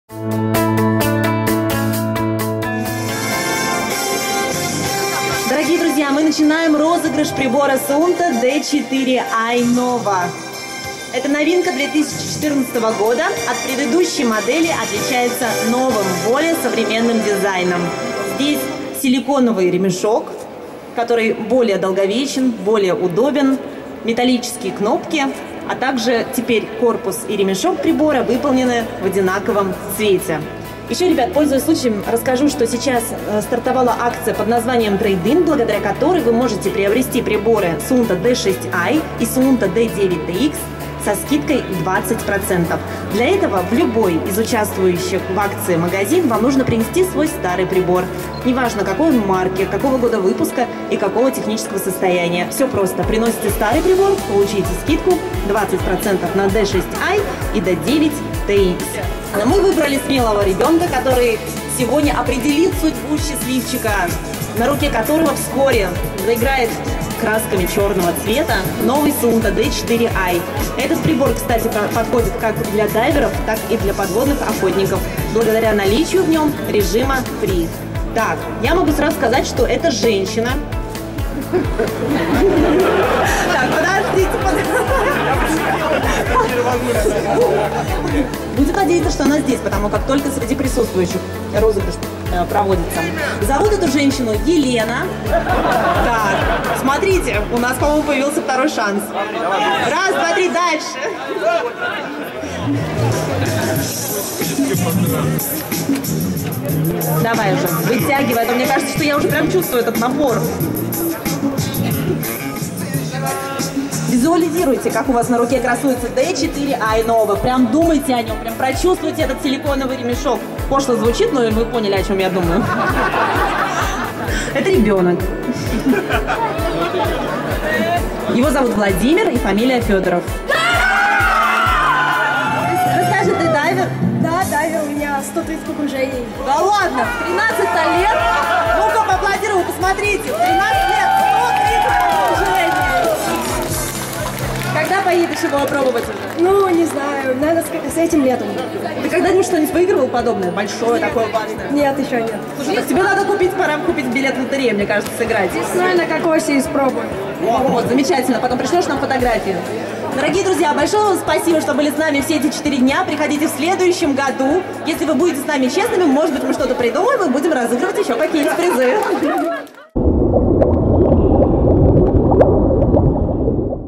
Розыгрыш SUUNTO D4i на Золотом Дельфине-2014
Традиционно на фестивале "Золотой дельфин" проводится розыгрыш дайв-компьютеров SUUNTO D4i - каждый день по одному прибору. В 2014 году на четвертый день выставки новинку от SUUNTO выиграл 13-летний дайвер Владимир Федоров.